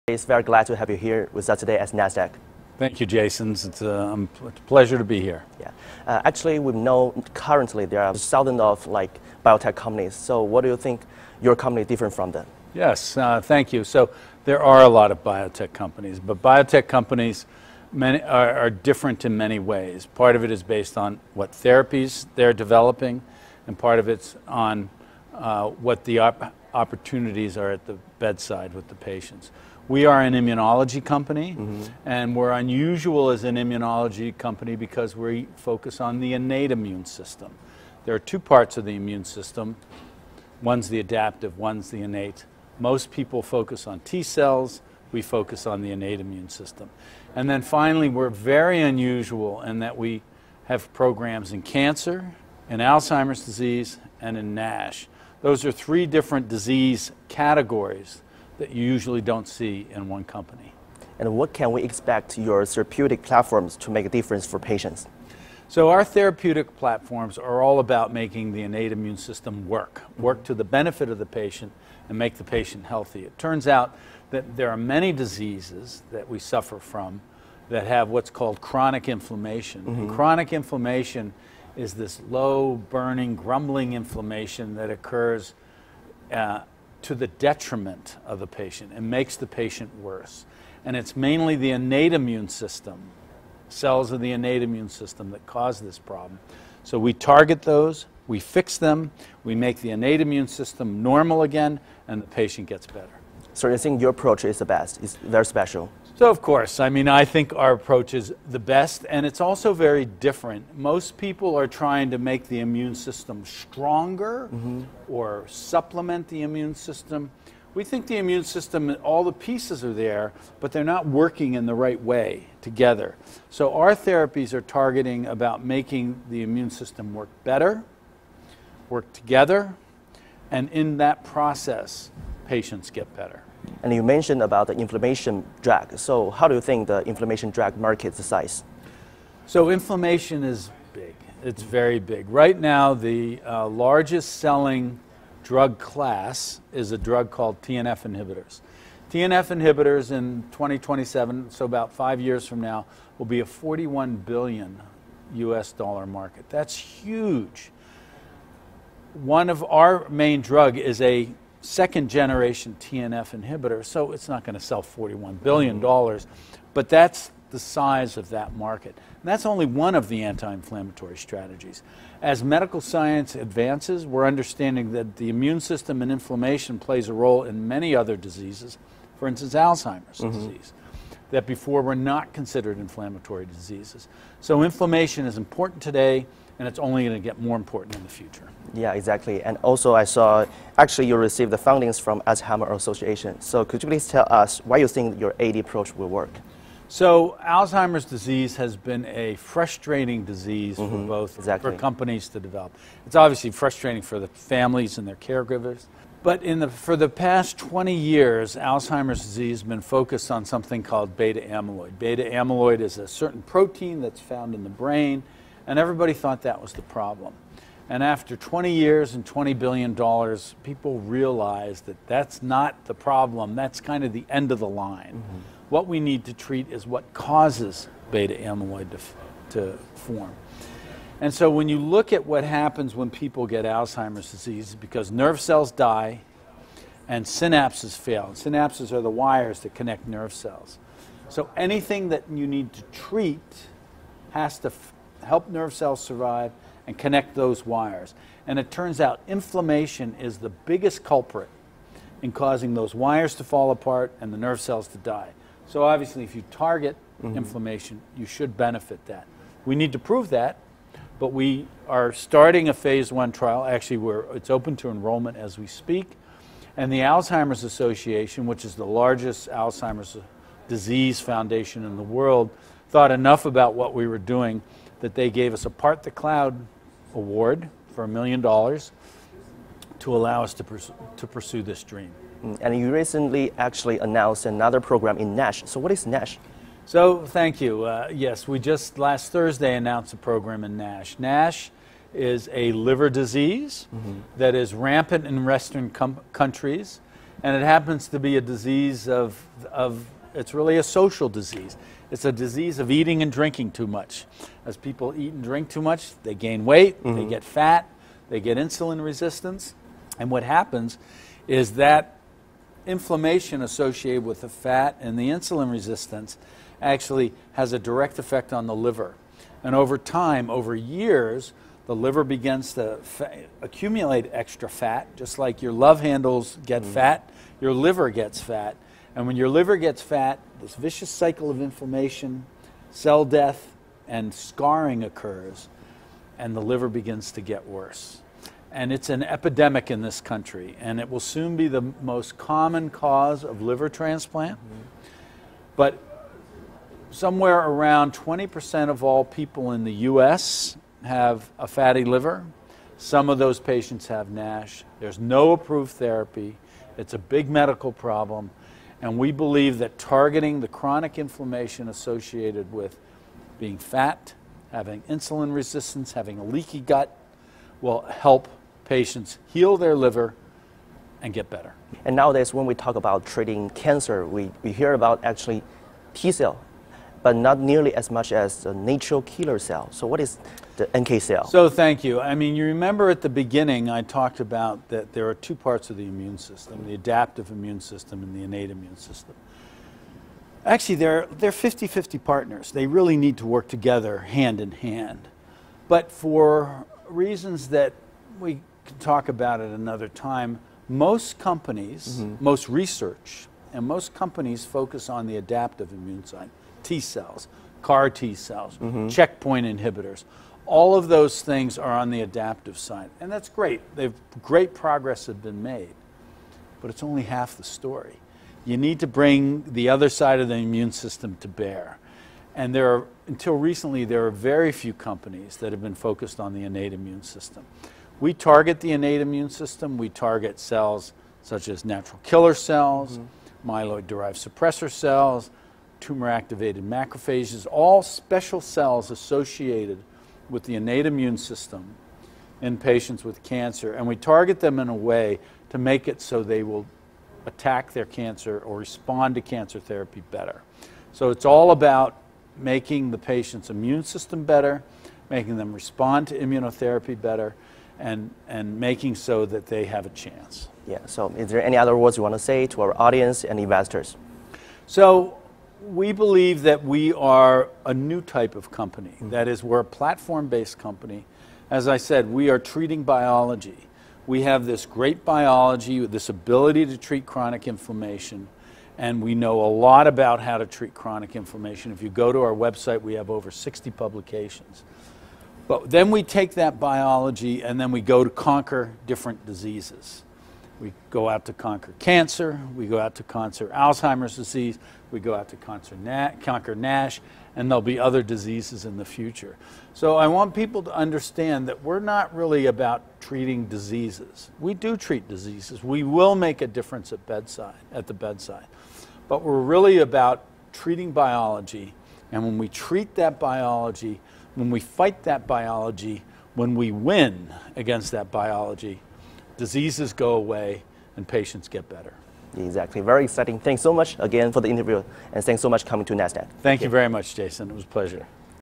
【CEO INTERVIEW| INmune Bio CEO/CMO Raymond J. Tesi, MD】
INmune Bio Inc. is a clinical stage biotechnology company that is developing new immunotherapies that reprogram a patient’s innate immune system to allow the immune system to fight cancer and Alzheimer’s. We just interviewed the CEO Raymond J. Tesi, MD at Nasdaq. Let's check it out.   20190920